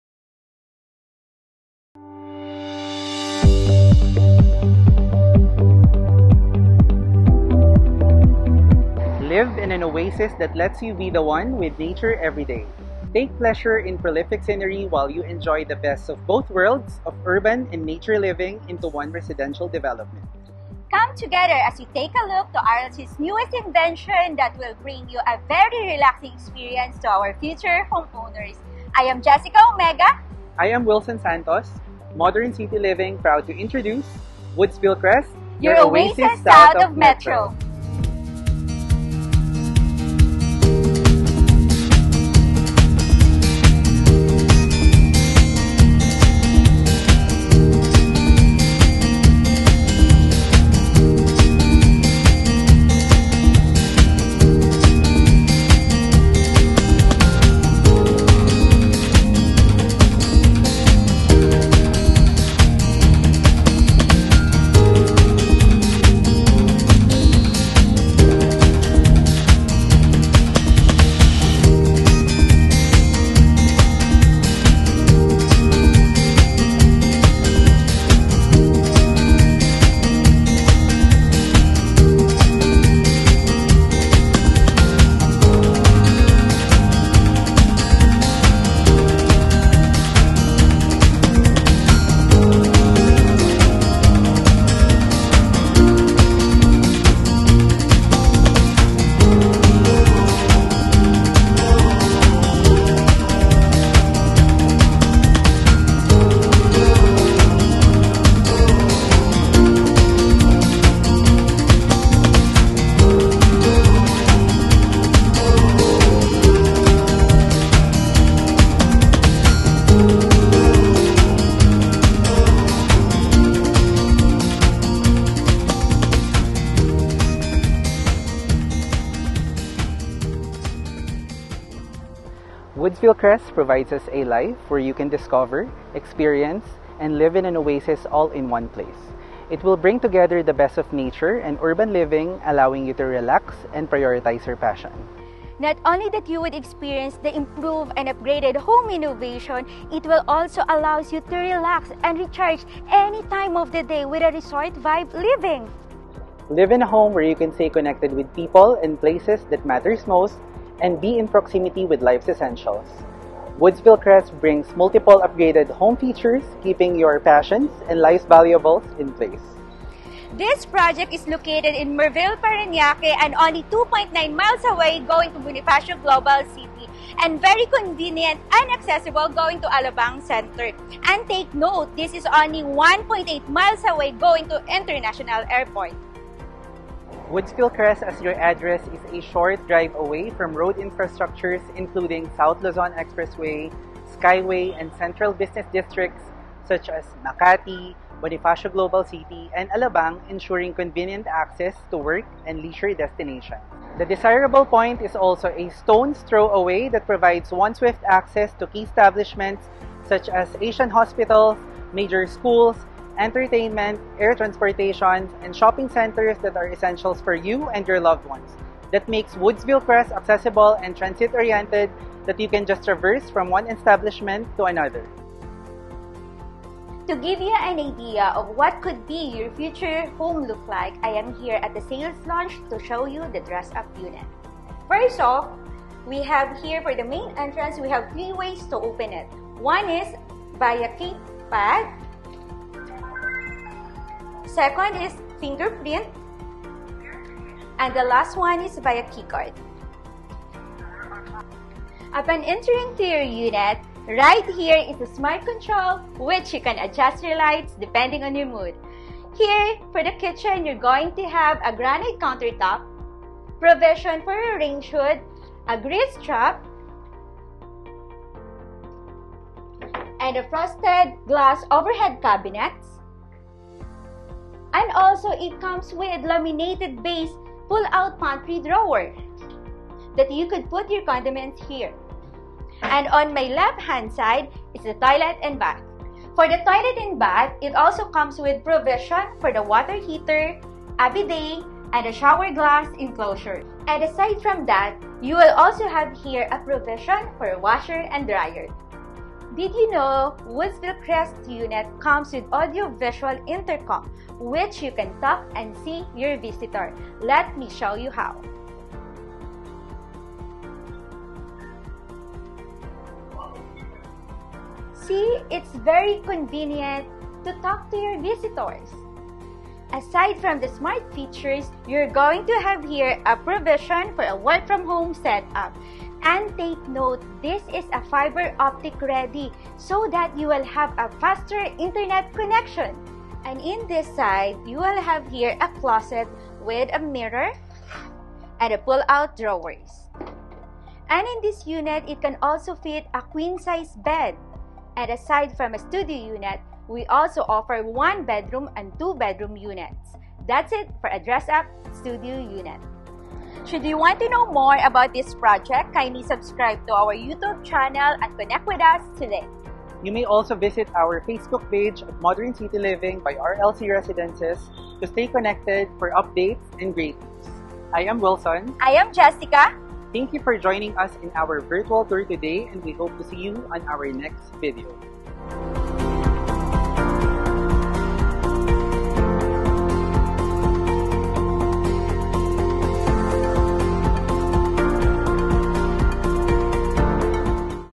Woodsville Crest - RLC's newest invention brings together nature-inspired development.
RLC's newest invention that brings together nature-inspired development, smart home features, resort vibe architecture and meaningful connections all in one place.

Investment starts at Php 9,000 monthly.